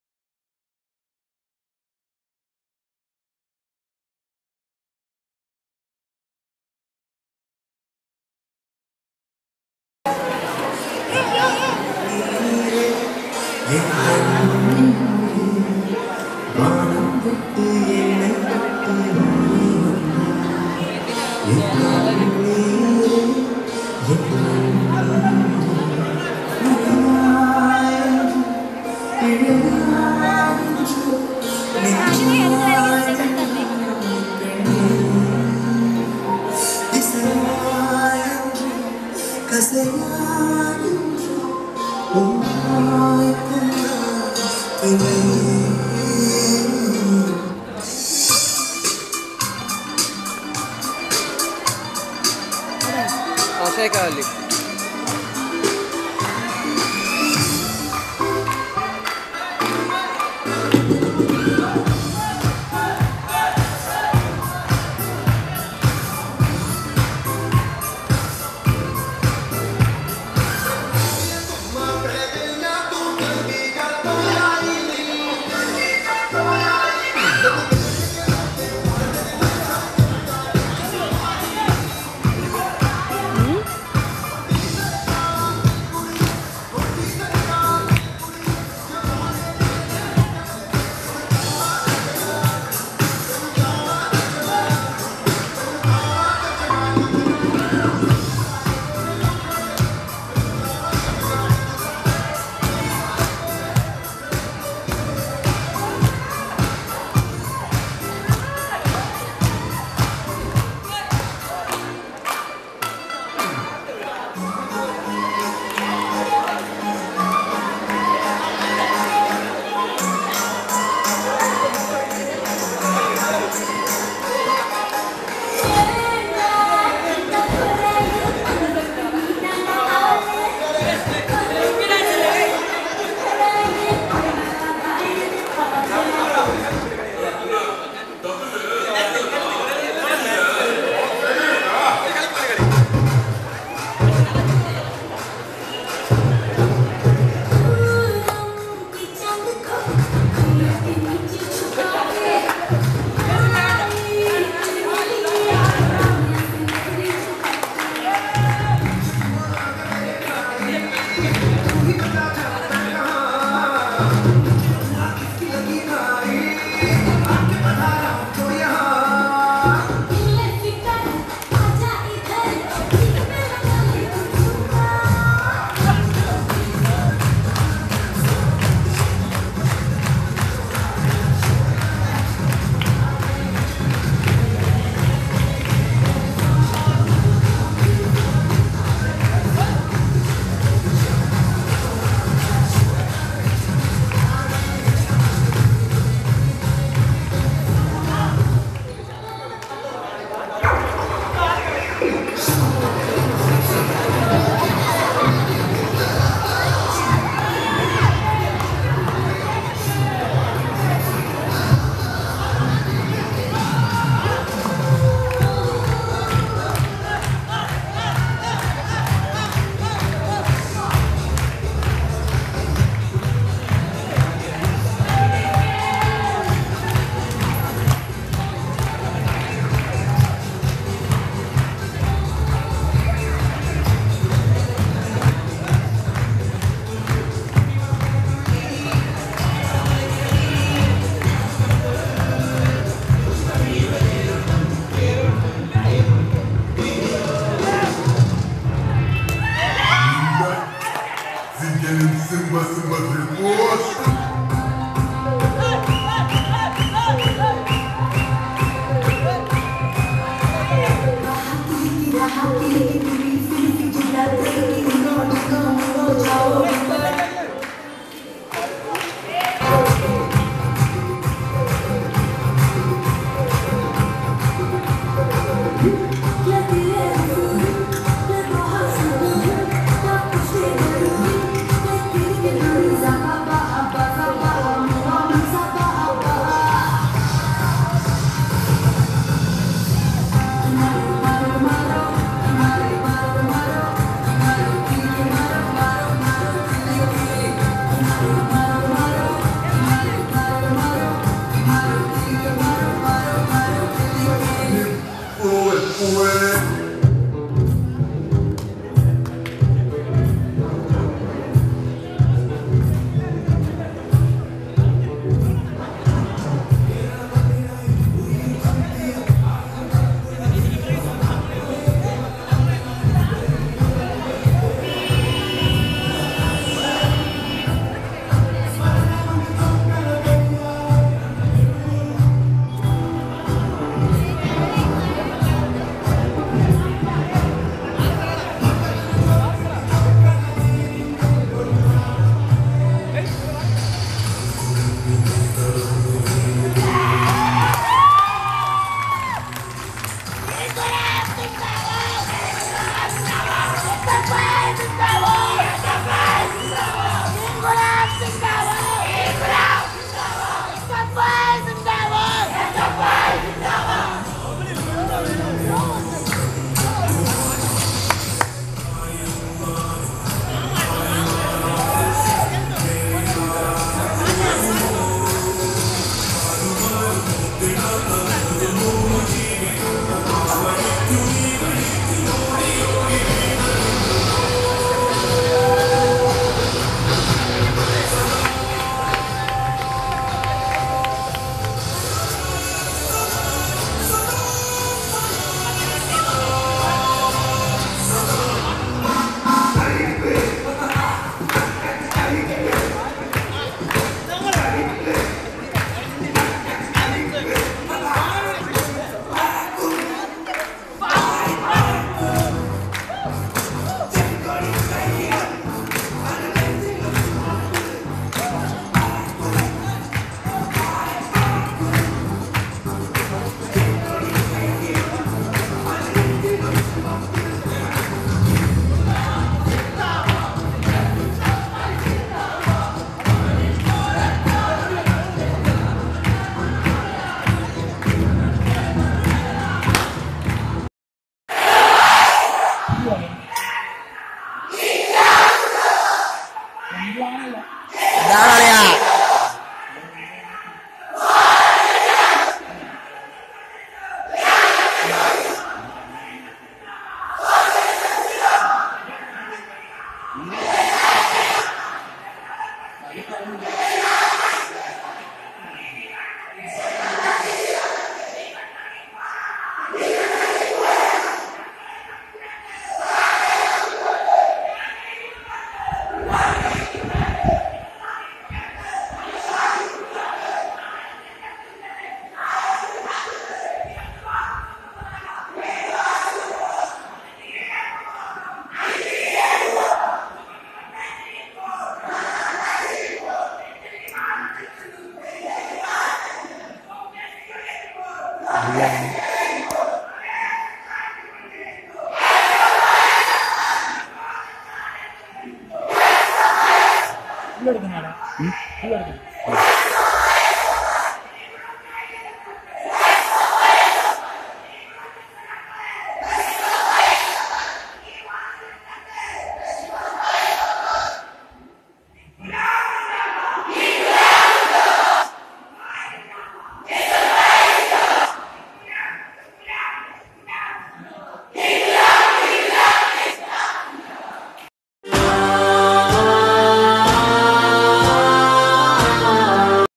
Flash MOB || SFI KNM UNIT 2k17
College students union fresher's welcome